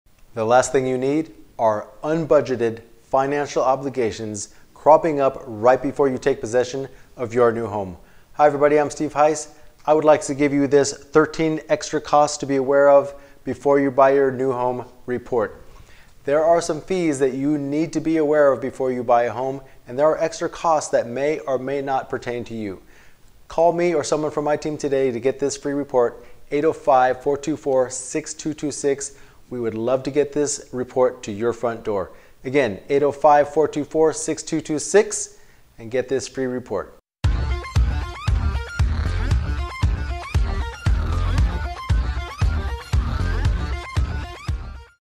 13 Extra costs to be aware of before buying a home
There are fees you may not be aware of when buying a home.  Get a free report from Steve and Tricia, to find out what will need to come out of your pocket before you take possession of your new home.

What will your home sell for? Free, quick online home evaluation.